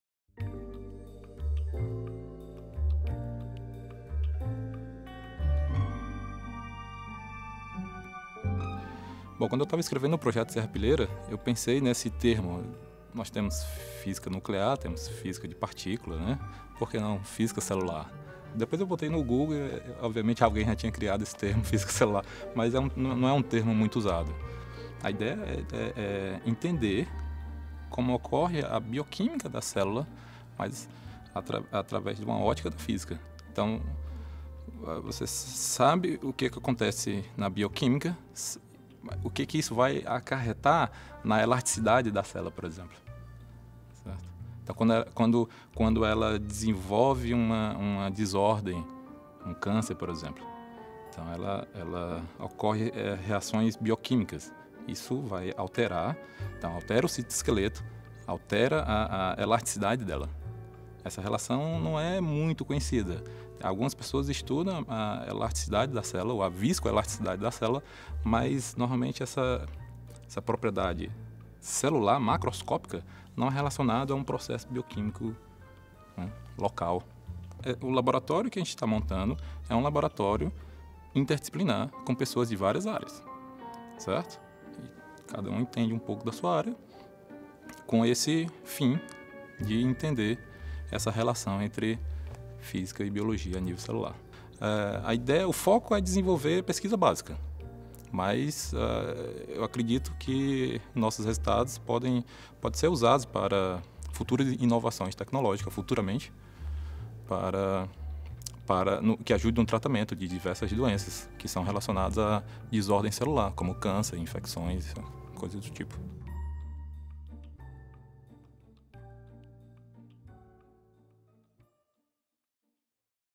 Por que a Física auxilia a Biologia a entender as células? | Episódio 54 (Nível III)
Desafiamos o físico Claudio Lucas a apresentar sua pesquisa para crianças, universitários e especialistas de sua área. Veja aqui a explicação para especialistas.

Físico da Universidade Federal do Ceará, ele estuda como as propriedades físicas das células dependem de seus processos bioquímicos, e como essas propriedades mudam com o surgimento de diferentes desordens celulares, ocasionadas, por exemplo, por doenças. A proposta é que os resultados de sua pesquisa possam servir como base para futuras inovações tecnológicas, incluindo o desenvolvimento de novos tratamentos para doenças relacionadas a essas desordens, como câncer e infecções.

CRÉDITOS:
Preparação de texto: Humberto e Maria Luisa Lange
Produção: Cadenza Filmes
Direção: Filipe Tomassini
Produção Executiva: Clara Queiroz
Direção de Fotografia: Micael Hocherman
Som direto: Dudu Falcão
Coordenação de pós-produção: Rafael Andreoni
Trilha Sonora: Marcelo Delamare
Edição: João Pedro Sá
Videografismo: Bruno Yoguy
Finalização e cor: Filipe Tomassini e Micael Hocherman